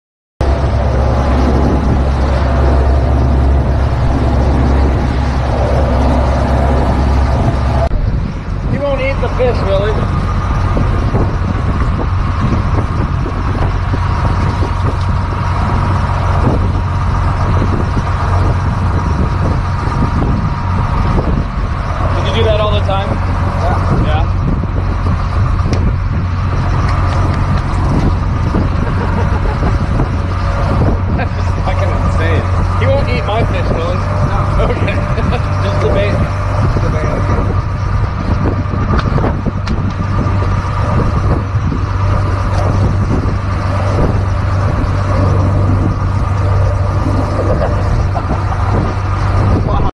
Gib meinen Fisch
Seelöwe kommt aufdringlich an das Boot